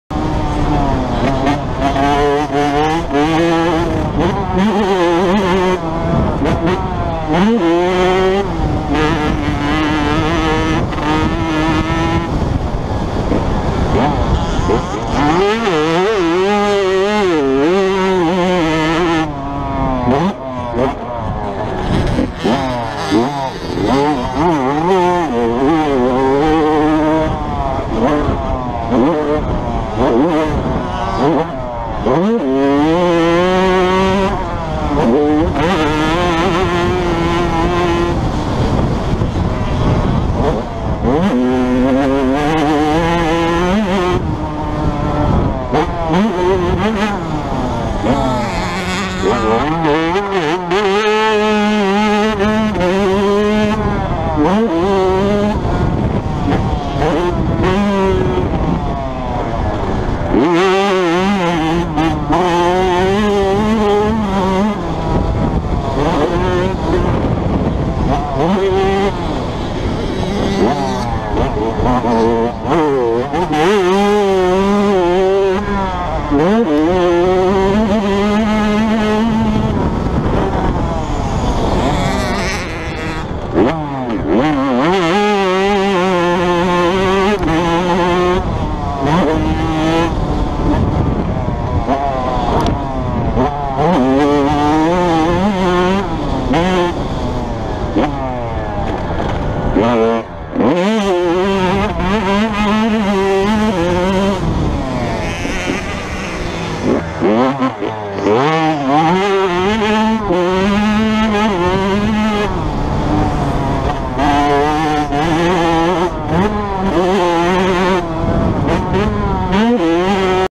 Carsyn Forcum Chillitown MX April 19 2015 Kawasaki 85cc
Few laps at Chillitown on April 19 2015